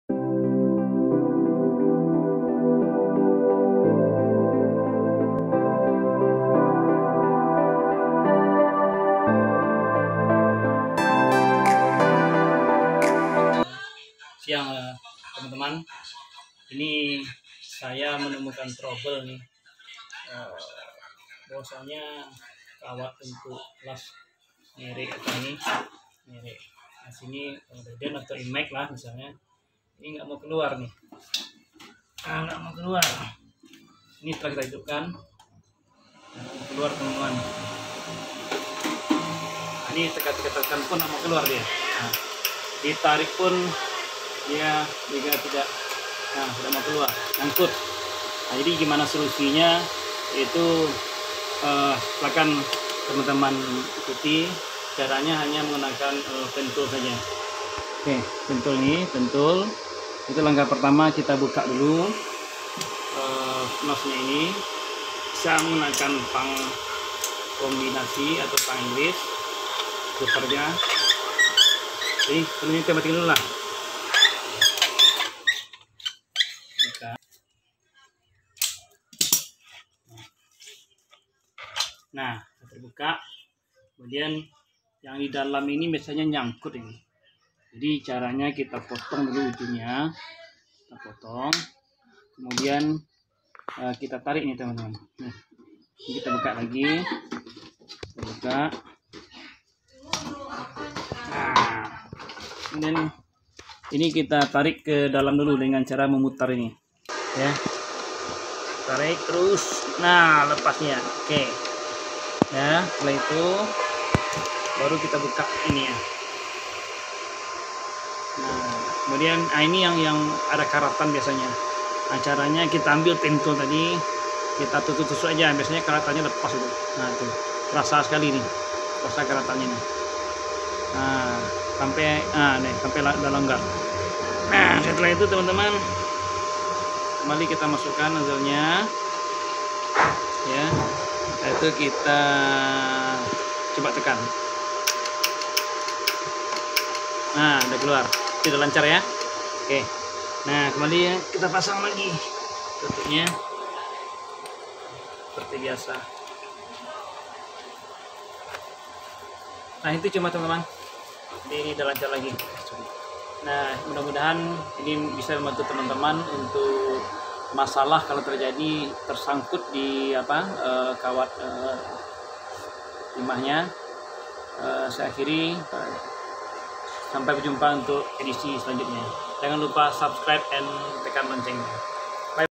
cara mengatasi kawat las macet pada nozle las image
cara mengatasi kawat las yang macet tidak mau keluar  pada nozle las image menggunakan peniti